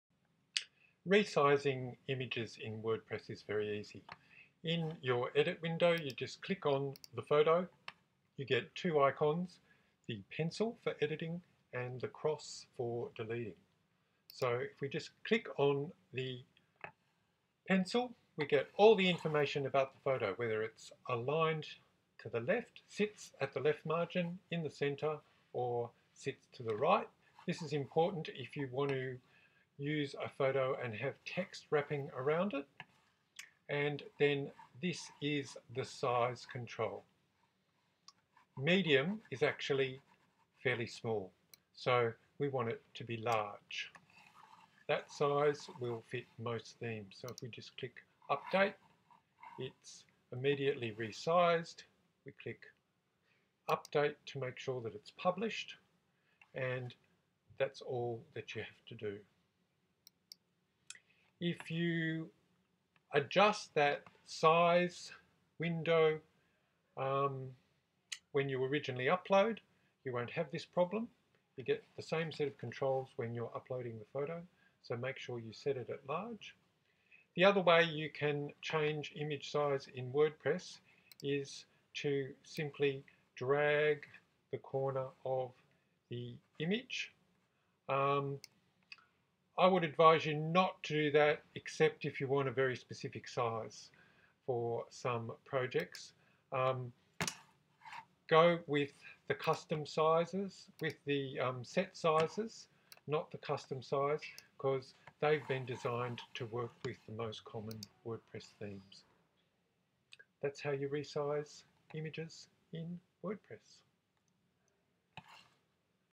Changing image size in Wordpress
How to change the size of your images in Wordpress 3.9